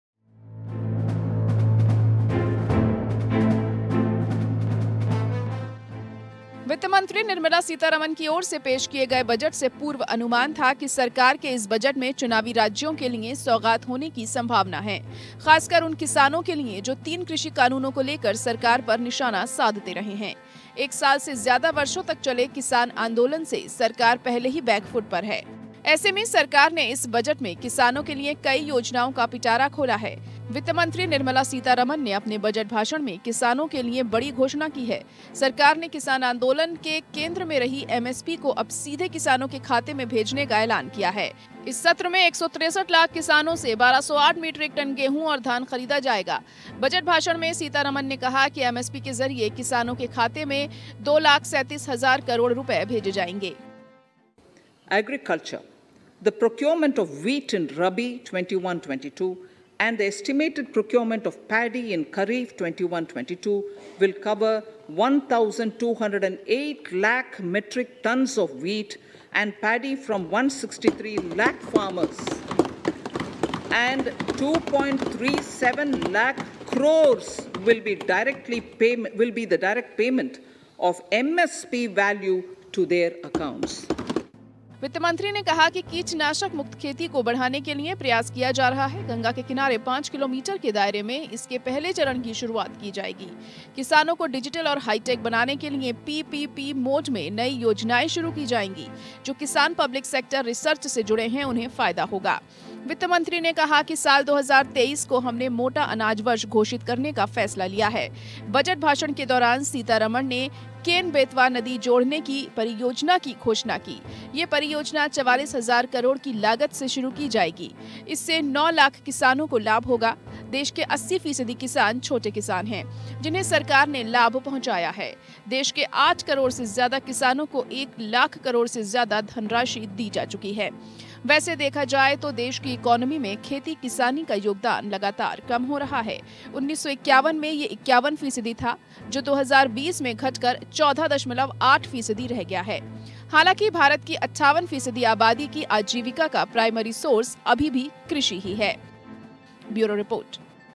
Budget 2022: MSP के जरिए अब सीधे किसानों के खाते में 2.37 लाख करोड़ रुपए भेजे जाएंगे
सरकार ने इस बजट (Budget) में किसानों के लिए कई योजनाओं का पिटारा खोला है। वित्तमंत्री निर्मला सीतारमण (Nirmala Sitharaman) ने अपने बजट (Budget)भाषण में किसानों के लिए बड़ी घोषणा की है। सरकार ने किसान आंदोलन (Farmers Protest) के केंद्र में रही MSP को अब सीधे किसानों के खाते में भेजने का ऐलान किया है। इस सत्र में 163 लाख किसानों से 1208 मीट्रिक टन गेहूं और धान खरीदा जाएगा। बजट भाषण में सीतारमण ने कहा कि MSP के जरिए किसानों के खाते में 2.37 लाख करोड़ रुपए भेजे जाएंगे।